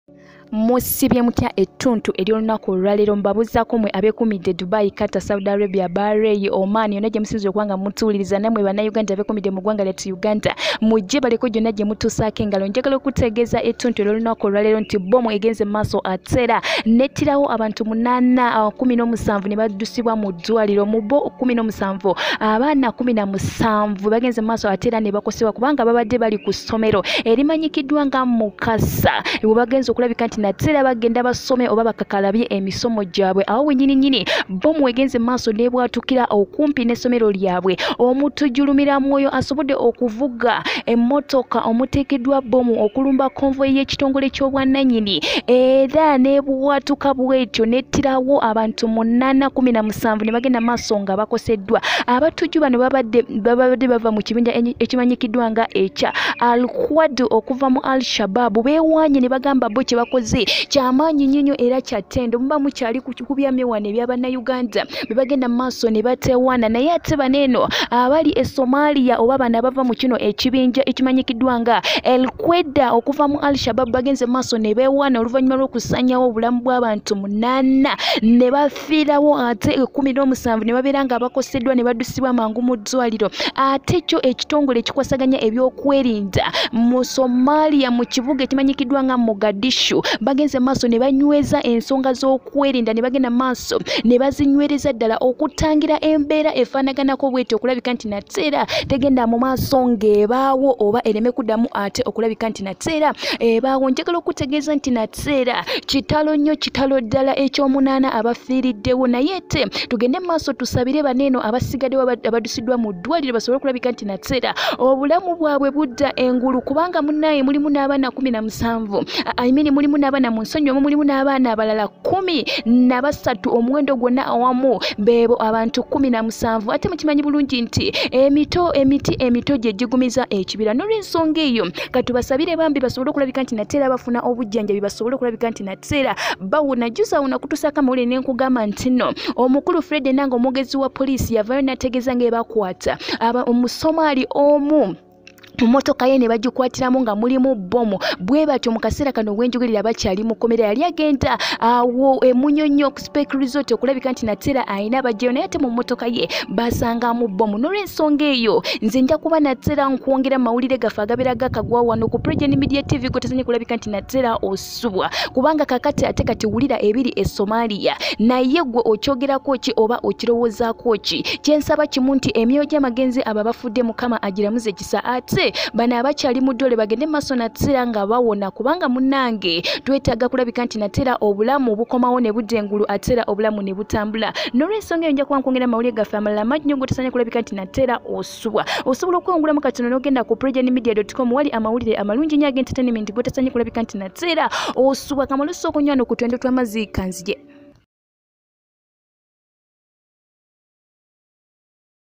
AMAZIGA! Bbomu esse 8, 17 batwaliddwa mu ddwaaliro nga bataawa omuli n’abaana, abatujju balabudde
Bbomu esse abantu 8 mu ggwanga erya Somalia mu kibuga Mogadishu, ekirese abannansi nga bali mu kutya enkya ya leero.

Abantu 17 banyiga biwundu mu kiseera kino, omuli abaana 13 abalumiziddwa, nga bali ku ssomero lya Mucassar mu disitulikiti y’e Hodan okumpi ne bbomu webwatukidde.

Omutujju, ‘Lumira mwoyo’ asobodde okuvuga emmotoka omuteekeddwa bbomu okulumba Konvoyi y’ekitongole ky’obwannanyi ekikuuma era mu kubwatuka kwe kutta abantu ssaako n’abaana b’esomero.

Wabula abatujju bakabinja ka al-Qaeda okuva mu kabinja ka al-Shabab, bewaanye okulemberamu okusindika bbomu, esobodde okutta abantu.

Mu kiseera kino ebitongole ebikuuma ddembe, byongedde okunyweza ebyokwerinda mu kibuga Mogadishu, okuyambako mu kutangira embeera y’emu okuddamu.